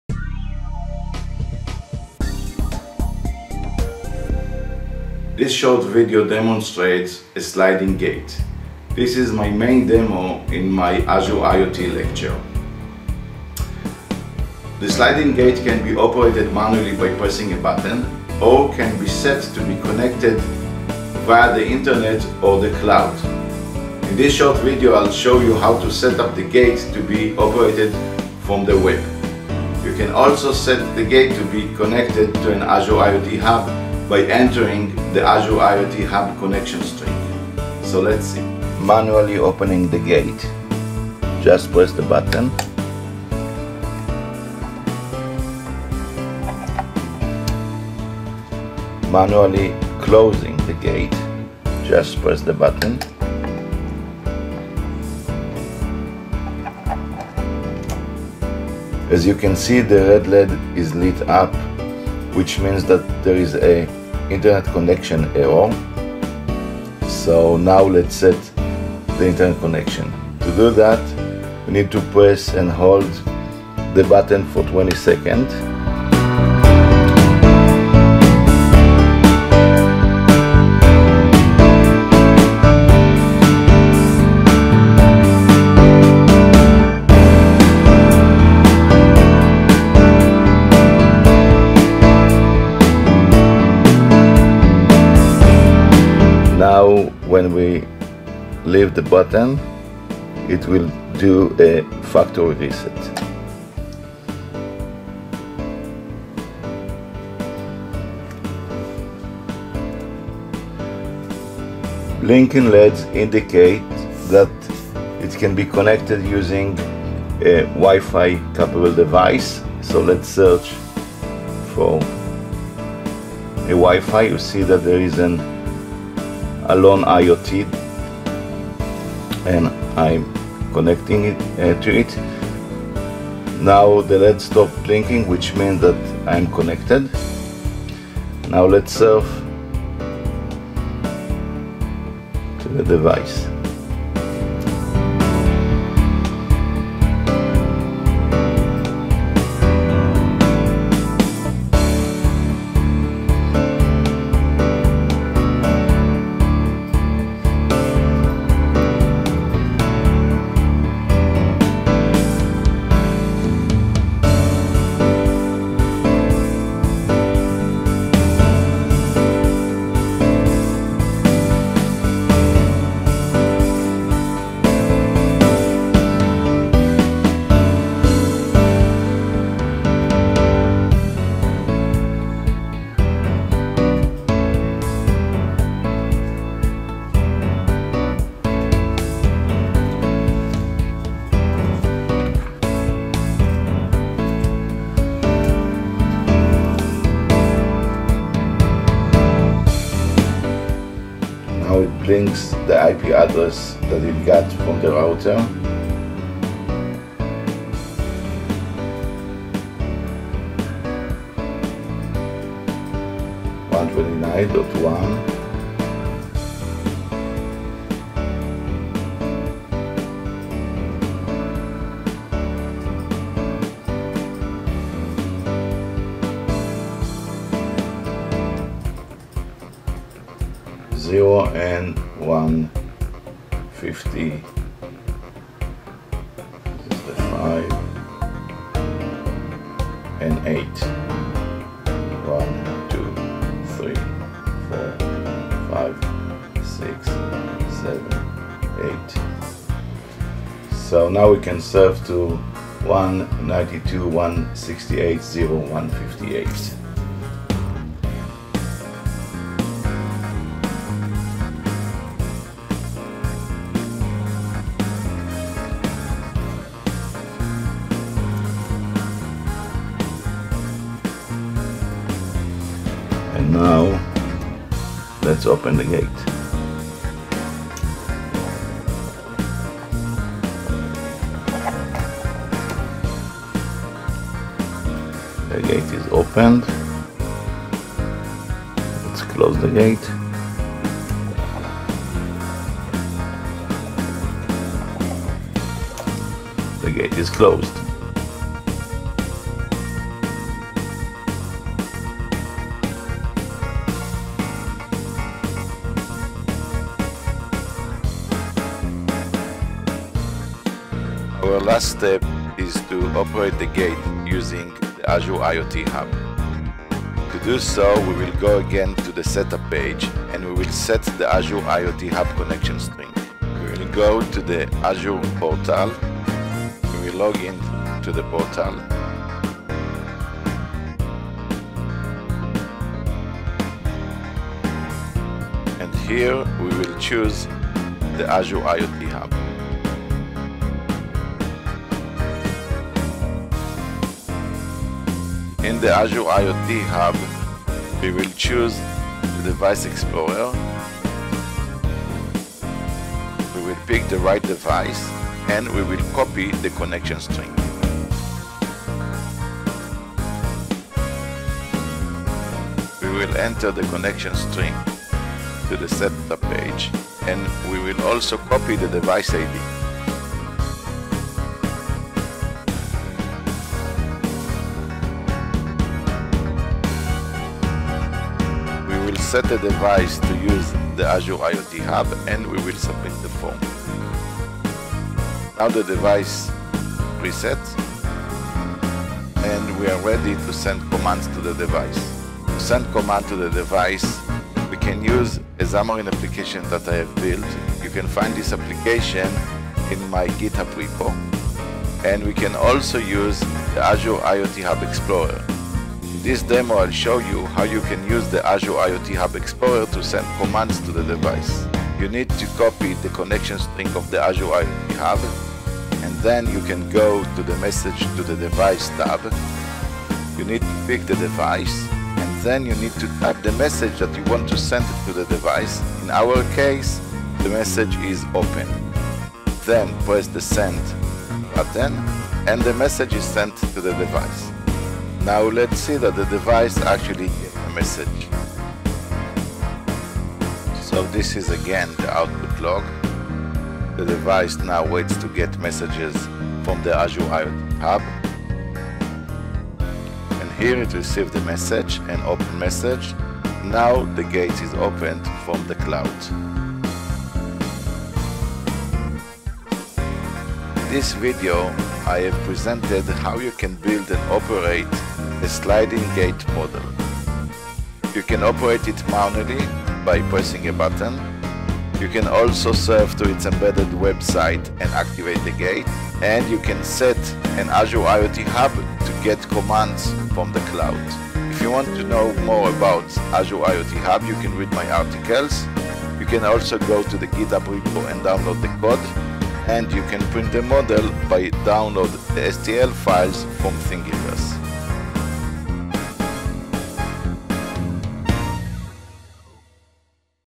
WebAndAzureIoTHubControlledDevice
A short video that demonstrate a sliding gate model that can be activated manually, remotely via an embedded web site or by utilizing Azure IoT Hub.